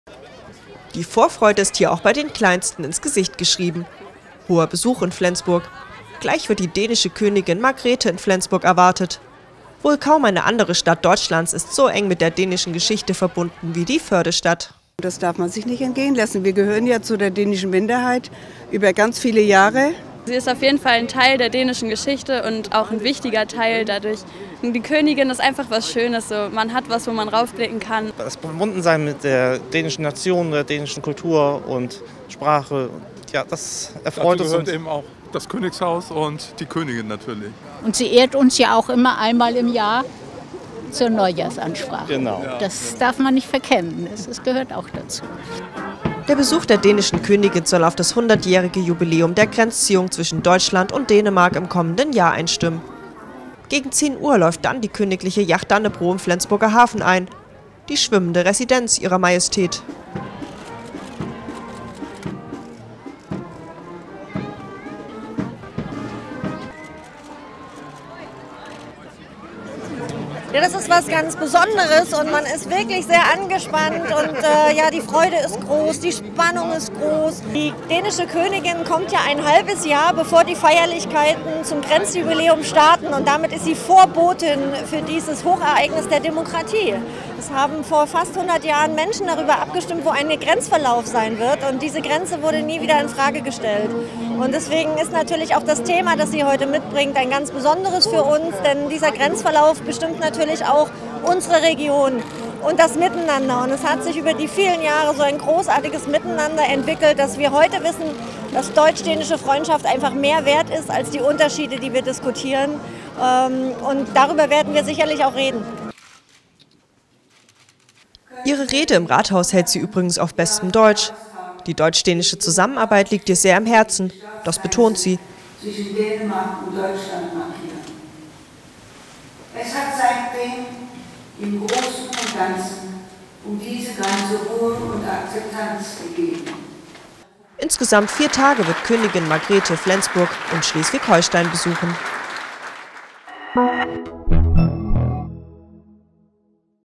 Majestätischer Empfang: Dänische Königin Margrethe II. besucht Flensburg
Flensburg bereitet seinem hohen Besuch einen festlichen Empfang. Gegen 10 Uhr erreicht die dänische Königin Margrethe II. Flensburg. Vier Tage wird die Monarchin verschiedene Einrichtungen in Flensburg und Umgebung besuchen.